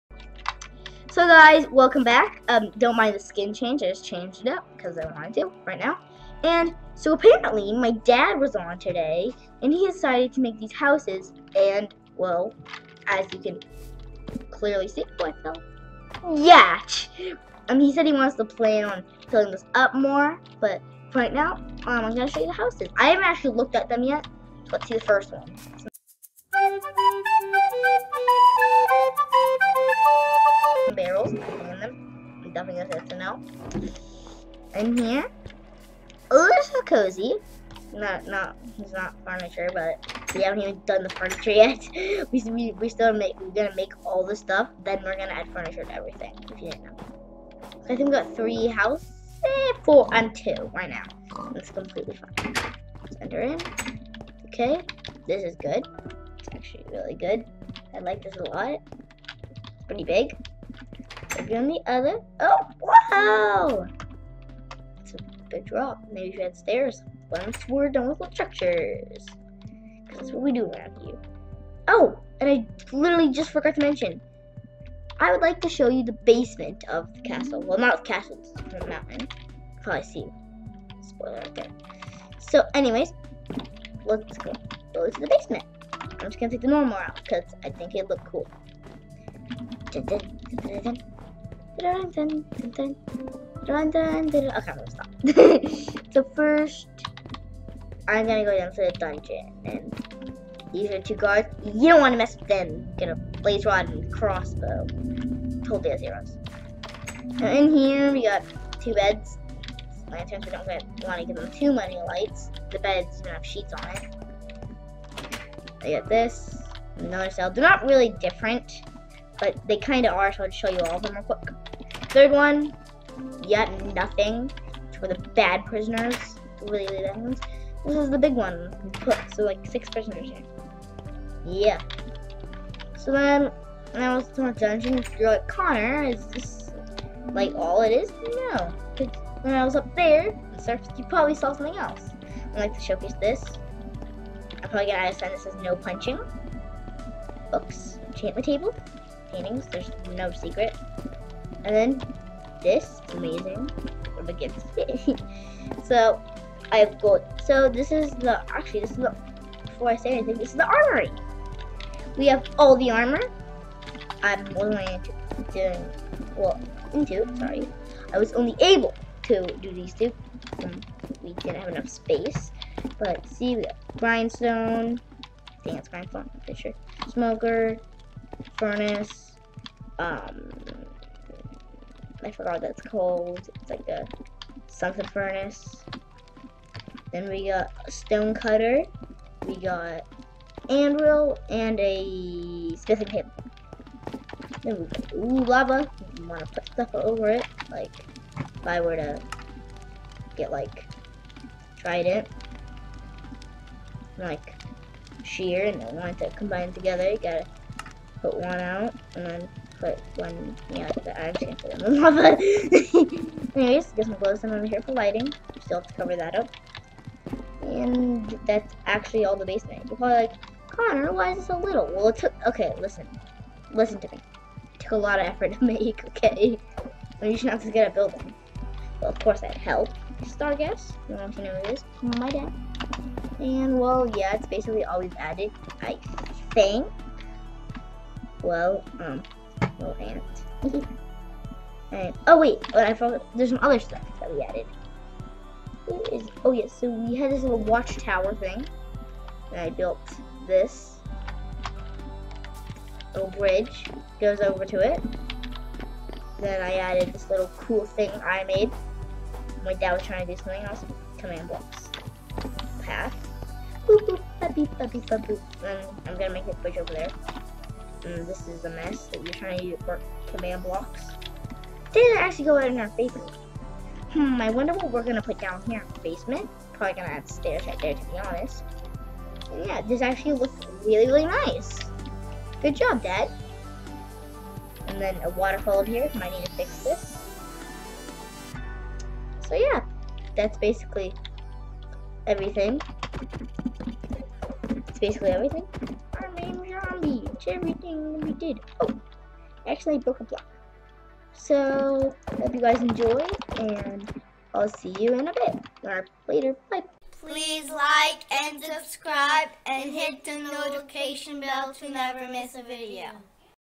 Building our Mountain Kingdom - Episode 3
I'm building a mountain kingdom No Name yet. My dad has been helping